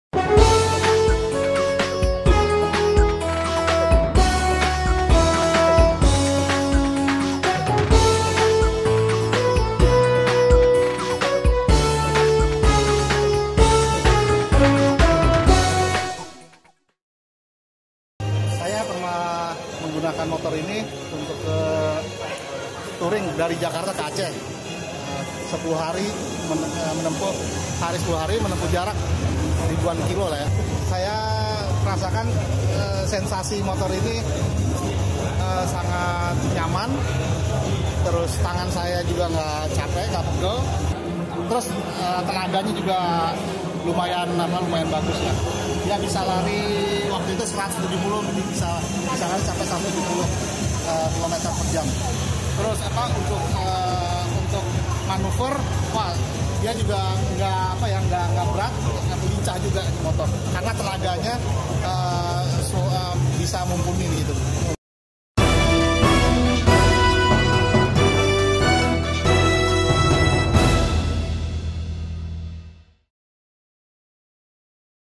Yamaha Tmax DX Cocok untuk Penggemar Touring
PT Yamaha Indonesia Motor Manufacturing meluncurkan skuter premium Yamaha Tmax DX di Jakarta, Sabtu, 24 Maret 2018. Sepeda motor ini didatangkan langsung secara Completely Build Up dari Jepang.